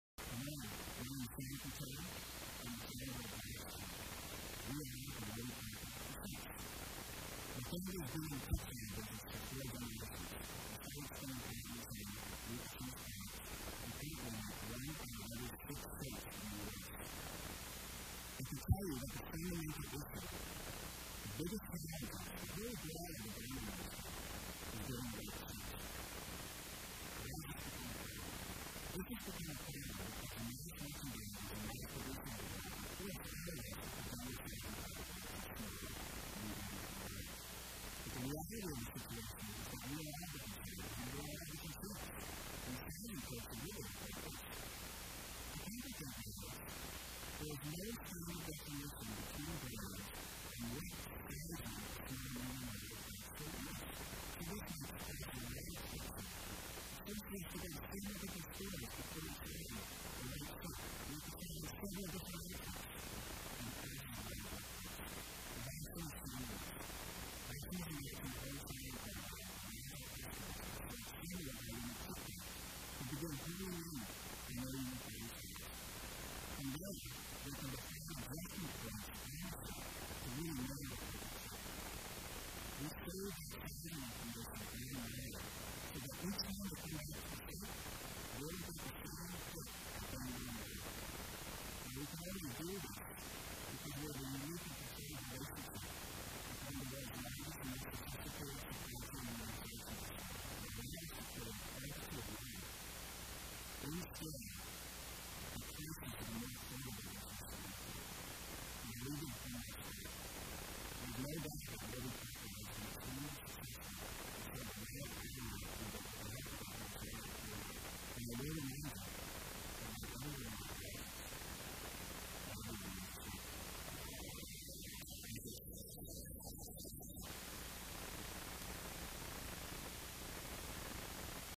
Vastrm Fashion | Warby Parker for Shirts | YCombinator Demo Day S12
We are warby parker for shirts. We started Vastrm to solve a basic problem: Shirts that don't fit well! Last face it, most off the rack purchases just don't fit right. Mass produced shirts were meant to accomodate a wide variety of body types with one standard size. Furthermore, different brands have standards for sizing. That is why many men must shop in 4 - 5 different stores before finding a shirt that actually fits! By providing a home try on program and giving the ability to make custom fit adjustments to all of our apparel, Vastrm will allow you to finally hone in on and find the perfect fit for your unique body type.